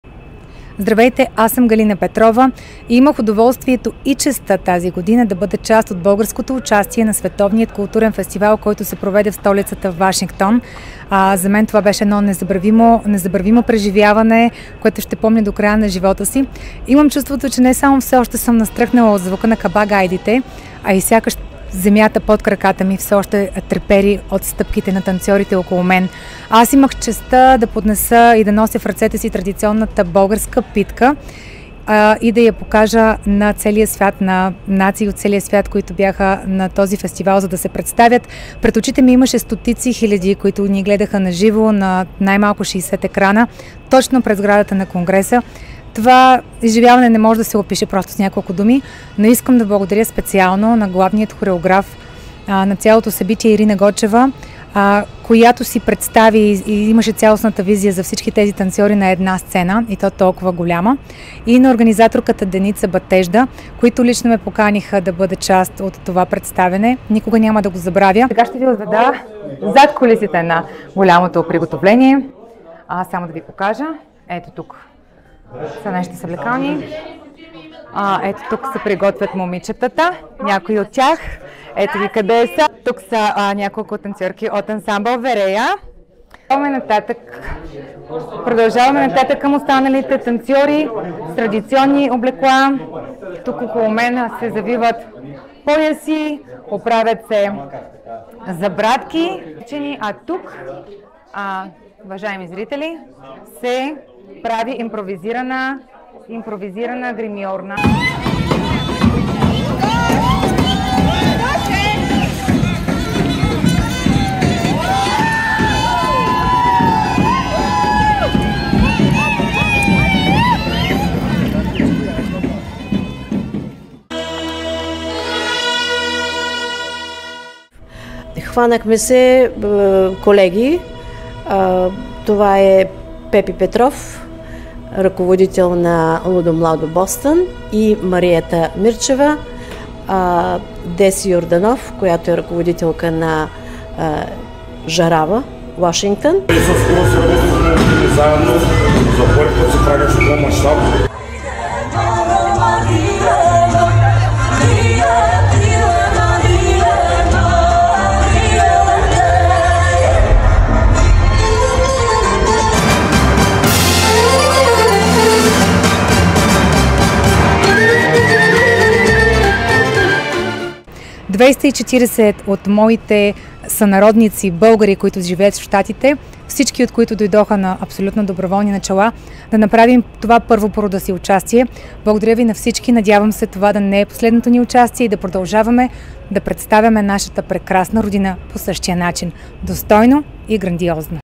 Зад кулисите на българското участие на World Culture Festival 2023 във Вашингтон
Вижте някои от моментите от посещението ни във Вашингтон като част от Световния Културен Фестивал Art of Living 2023 във Вашингтон.

В българската група взеха участие танцовите ансамбли: „Хоро“, Чикаго с ръководител Ирина Гочева, „Верея“, Чикаго с ръководител Константин Маринов, „Лудо младо“, Бостън с хореографи Пепи Петров и Мариета Мирчева, „Жарава“, Вашингтон с хореограф Деси Йорданоф и „Българе“, Калифорния.

Музиката за българското участие е създадена от популярния български композитор Георги Андреев, а подготовката протече с пълна сила месеци преди събитието. Всички участници репетираха по места чрез видео връзка, за да се осъществи синхрон между тях.

Главен организатор на това българско участие във Фестивала е Деница Станчева от Бостън, която е присъствала на всичките предишни издания на фестивала досега. Популярен с това, че събира хиляди участници от цял свят. (През 2011-та година 2000 българи се хванаха на хоро на олимпийския стадион в Берлин на същия фестивал)